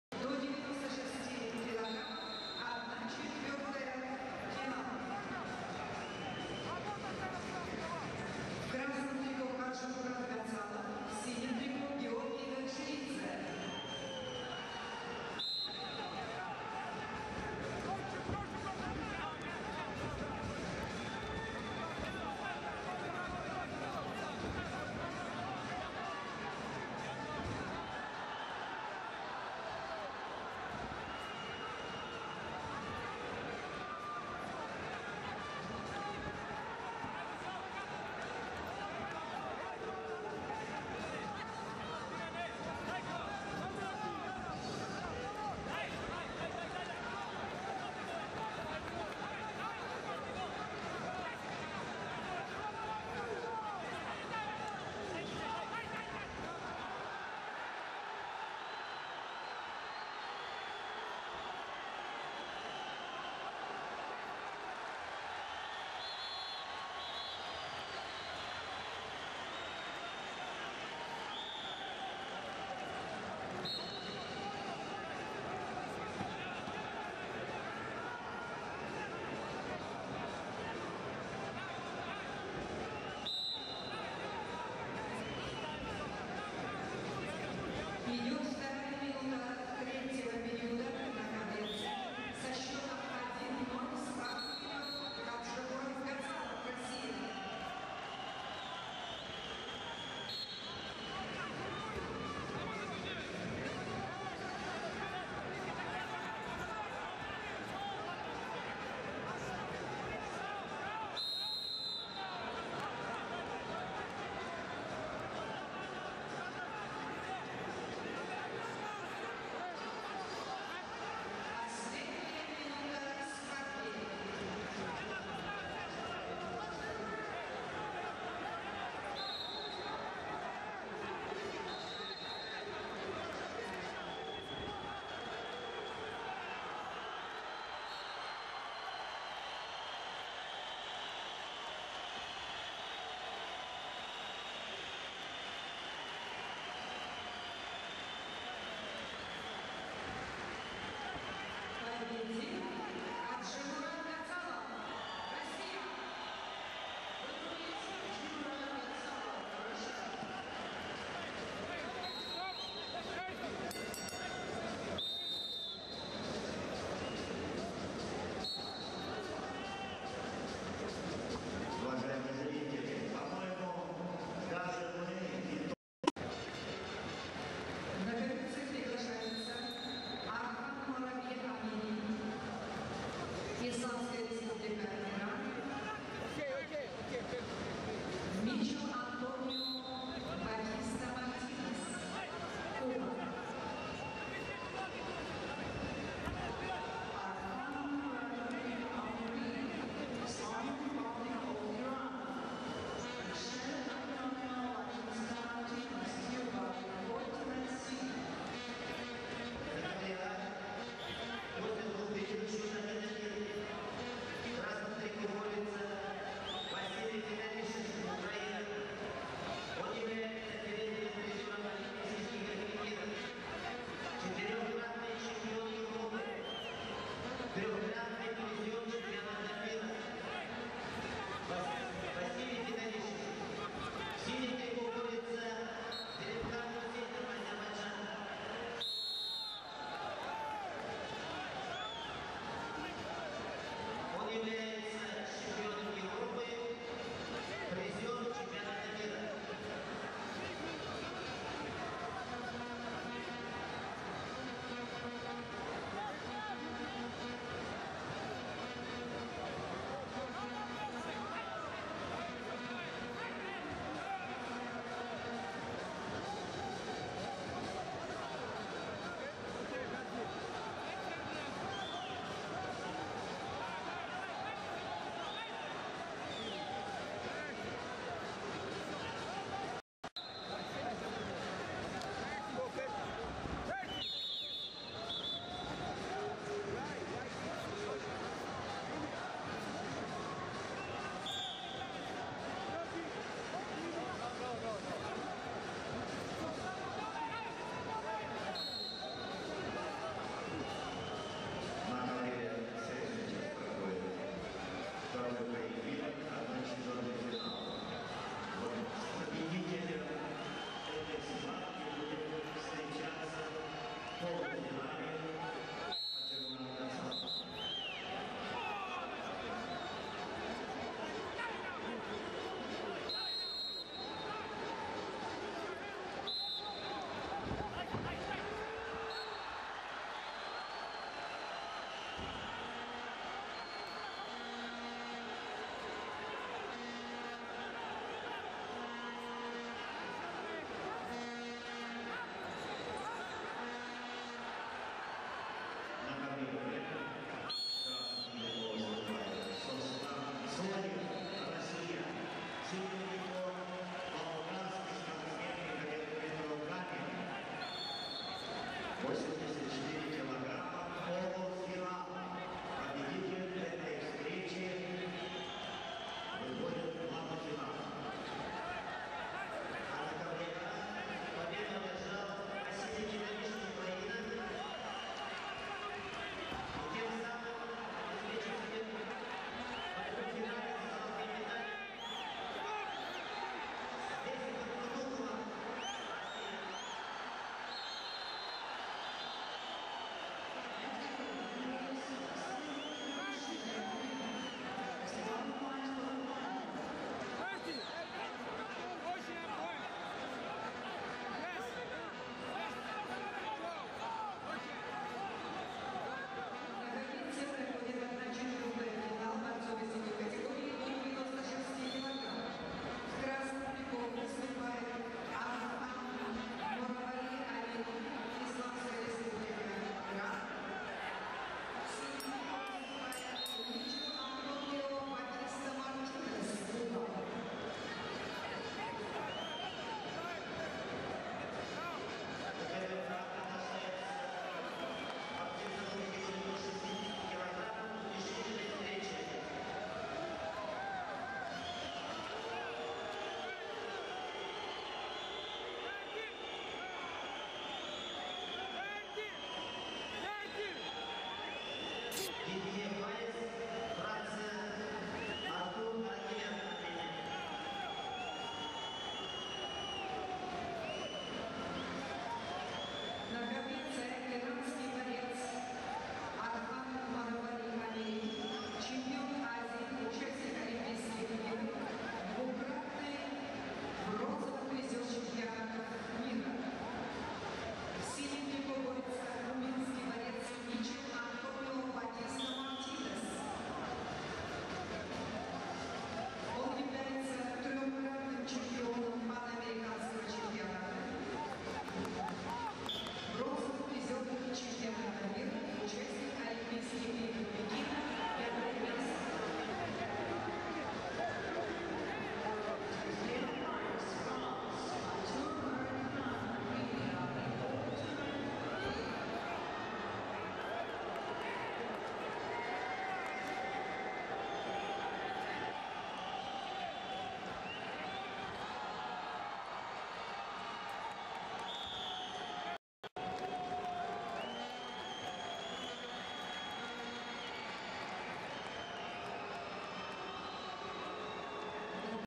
KTSOEV Soslan (RUS) -- GANEV Mihail Petrov (BUL) 1/2 Finale
FS - 84 Kg / World Championship - Senior / Moscou / 2010